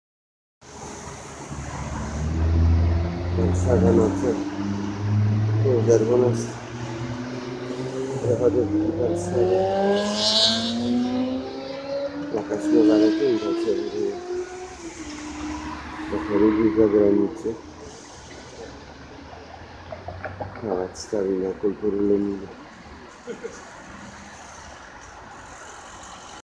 DAMY RADĘ,GDY MAZUR ZA DORADZANIE NA STRONĘ PEPICZKÓW PRZEPĘDZIMY I PO LICHWIE!!!
Weeeeeeeekkend w Warszawie w toku,ale nikt nie chce teraz religii,nawet poznawczo. ,ktos to wykorzystal i steruje teraz motlochem,calymi grupami tubylcow,aby dalej zachowywali sie nieprzyzwoicie w DNIACH OSTATNICH TEGO,ZLEGO SYSTEMU RZECZY pytajac sie zwykle slowem CO?Swiadkow Jehowy juz tu w Warszawie nie ma i nie chca przyjezdzac majac swoje religijne systemy zle.